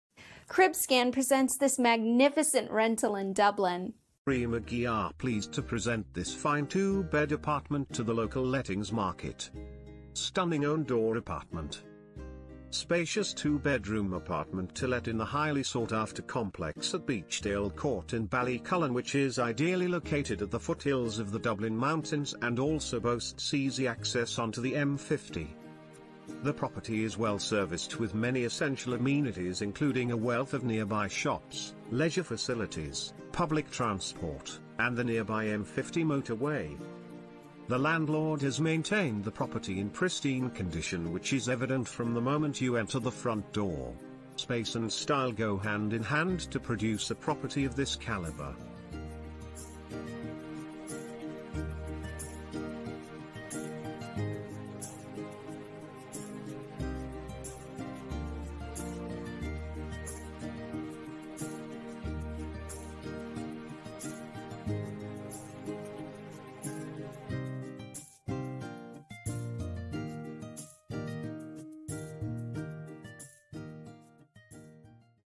Apartment to rent in Dublin, Oldcourt, €1,700 / monthly
2
1
Central heating

REF55533
REA MCGEE ARE PLEASED TO PRESENT THIS FINE 2 BED APARTMENT...